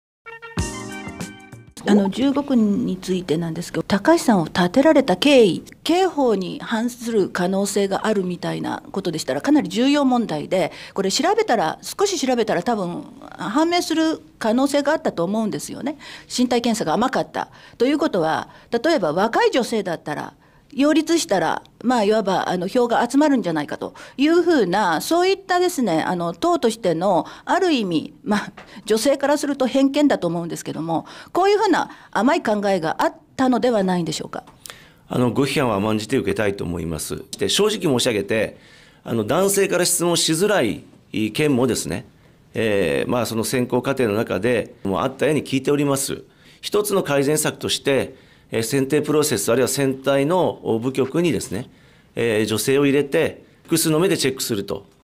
迷言・名言集【国民民主党代表 玉木 雄一郎編⑯】フリーの安積記者のキツいツッコミｗ「若い女性だったら...」
衆院東京15区補欠選挙で、国民民主党が公認候補予定者としていた高橋茉莉氏の公認取り消しの件について、フリーランスの安積明子記者は「若い女性だったら...」と、国民民主党の候補者選定そのものが間違っているのではと質問。
国民民主党の玉木雄一郎代表は「ひつとの改善策として...」と答えましたが、ちょっと的はずれな感じが...

また動画は別のサイトに転送され、タイムスタンプが追加されました。

･*:.｡ ｡.:*･ﾟ✽.｡.:*・ﾟ ✽.｡.:*・ﾟ ✽.｡.:*・ﾟ ✽.｡.:*・ﾟ ✽.｡.:*

なんちゃって【政治系Vtuberいなみ】へのアクセス⏬⏬⏬